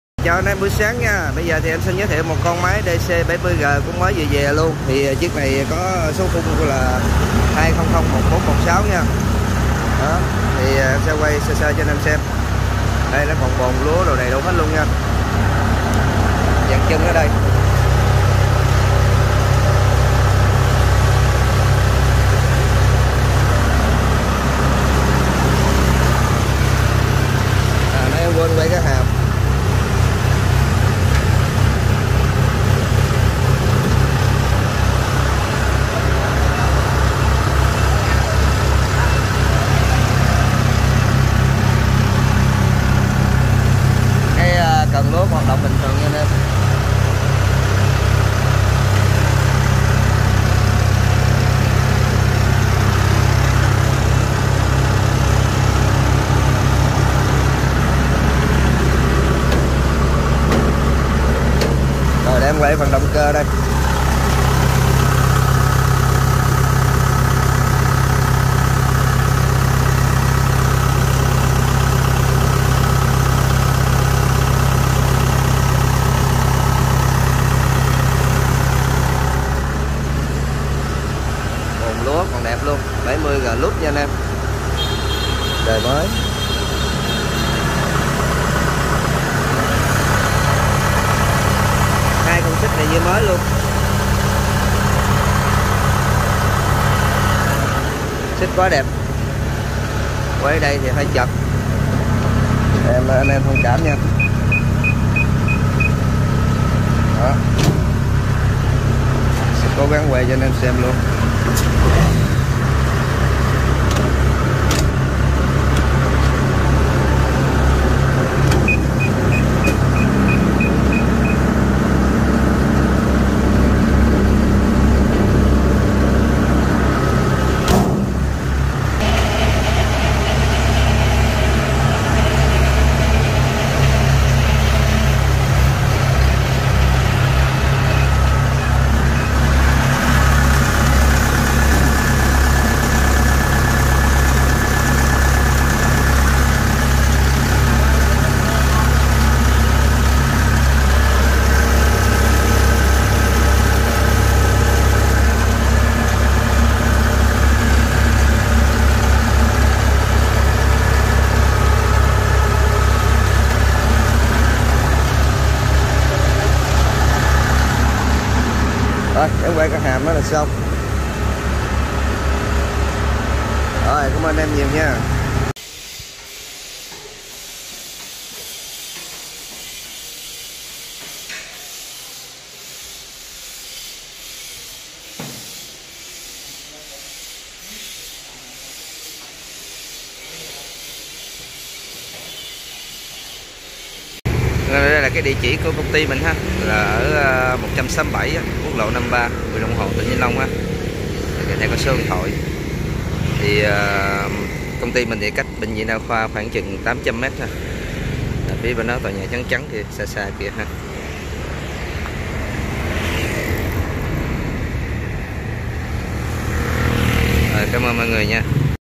201416 Test máy gặt lúa KUBOTA DC70G đã qua sử dụng còn 80% - 0839000100
201416 Test máy gặt lúa KUBOTA DC70G đã qua sử dụng còn 80% - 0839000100